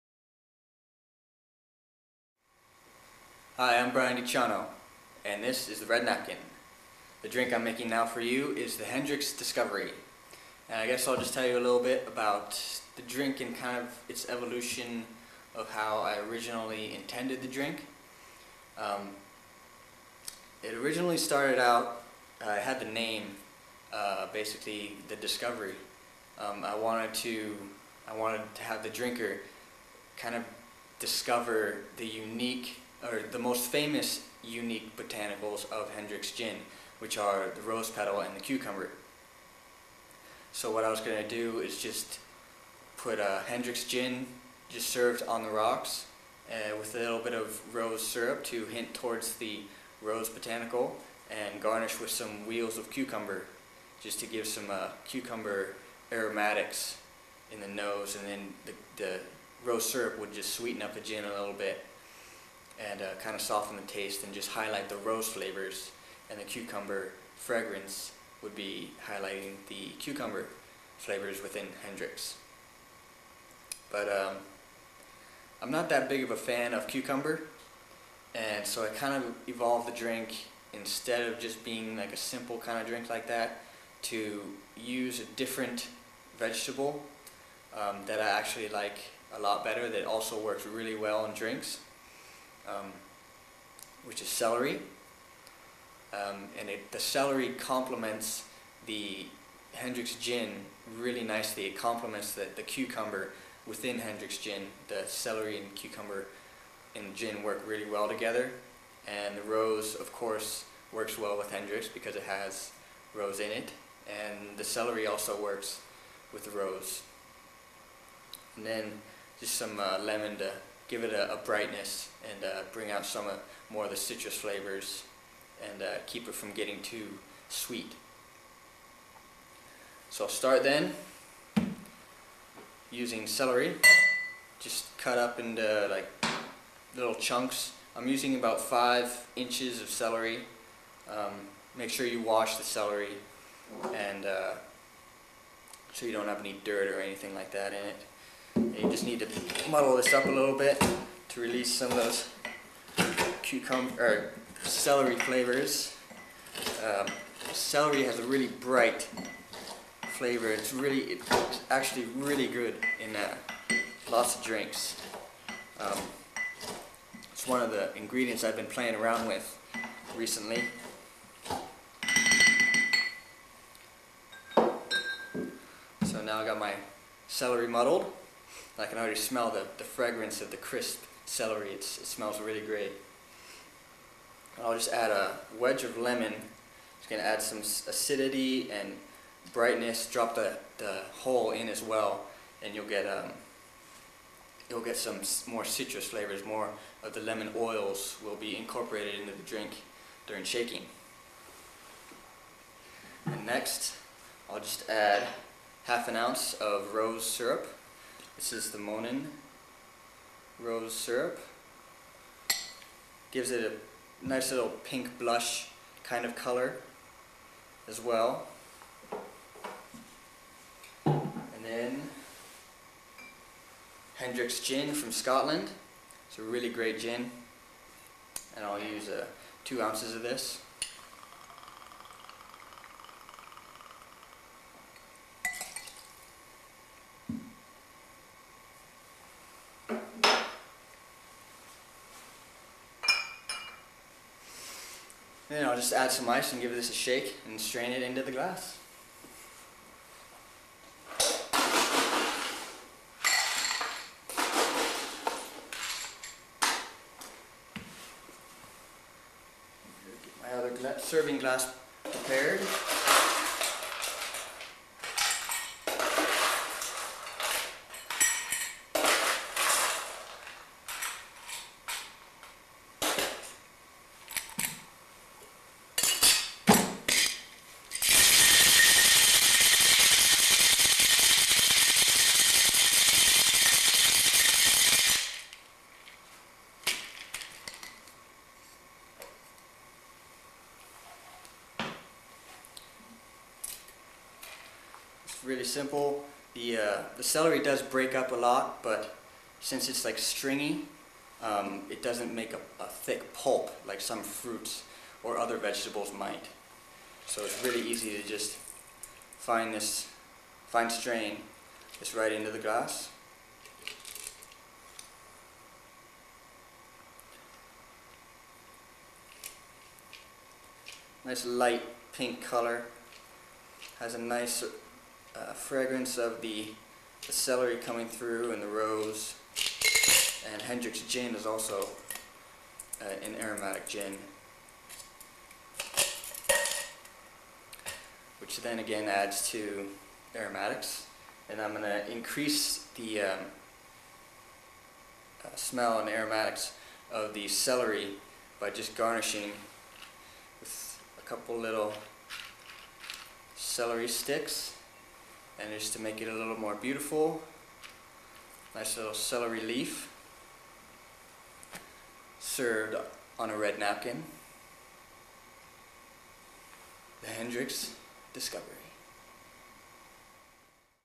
The Red Napkin #31 - Hendrick's Discovery
Discover the refreshing flavor of the Hendrick's Discovery as a spring and summer cocktail.